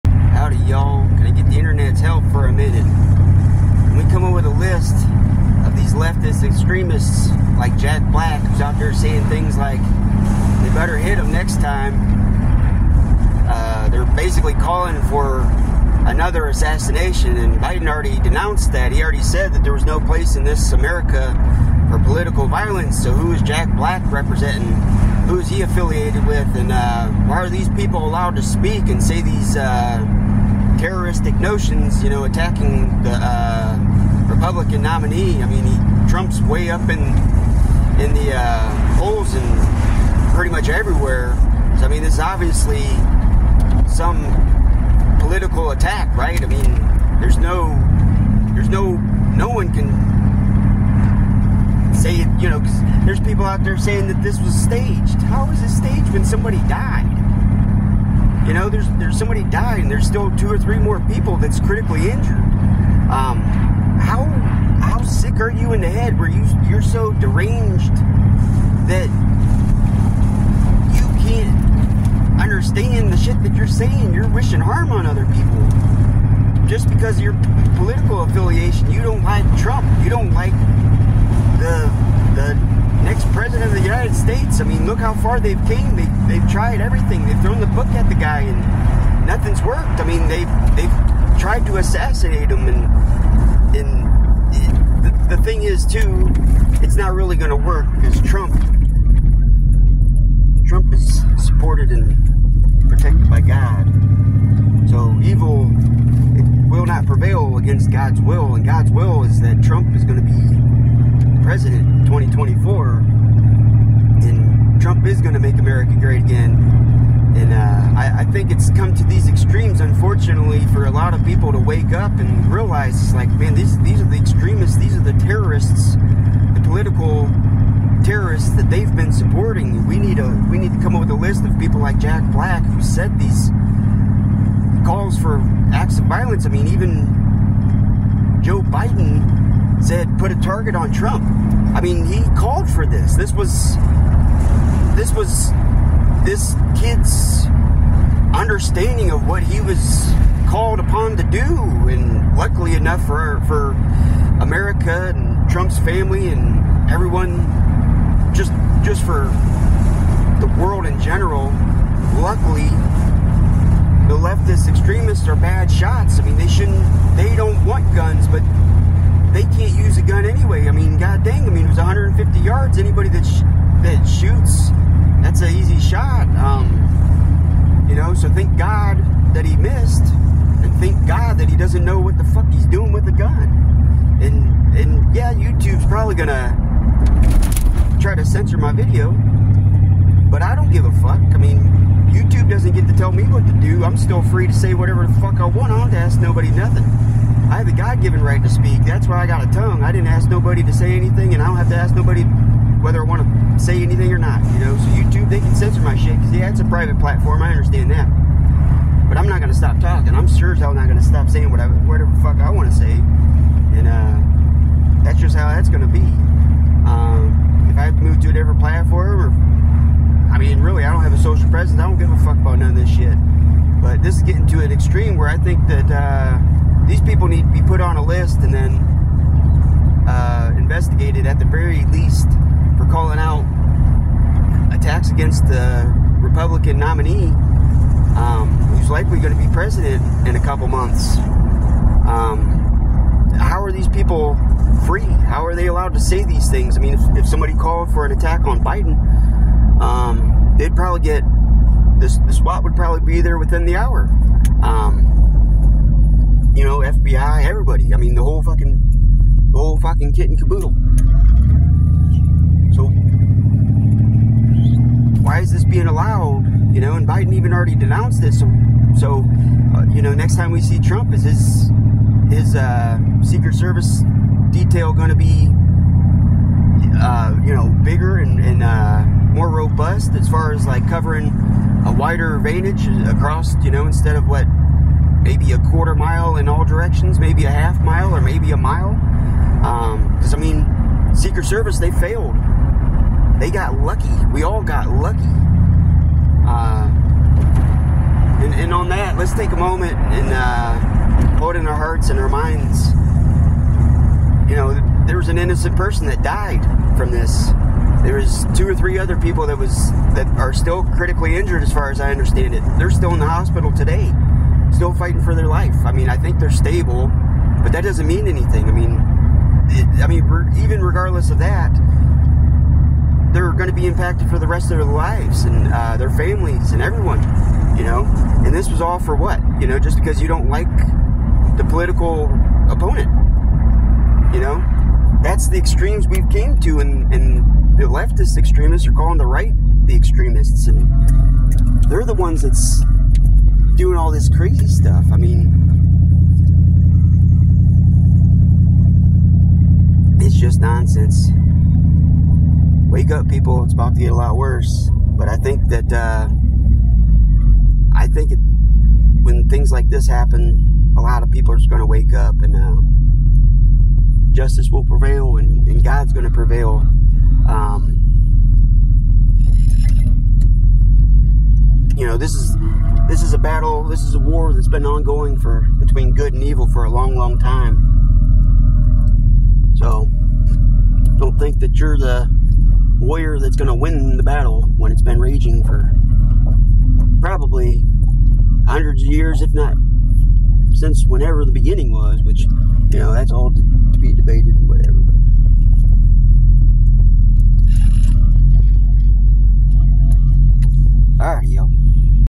Help me make Leftist Extremist List of people calling for Political Assassination in America
Political Terrorist Extreme Leftist like Jack Black should be under investigation for calling fir Political Assassination of Donald Trump.

Tho biden said himself to put "bullseye" on Trump, he said afterwards that Political Violence has no place in America. Pick a side sleepy joe.

They are terrorist of America attacking We the People by trying to assassinate Trump.

Please help make a list in the comments,  all the the people from various platforms who called for Political Violence.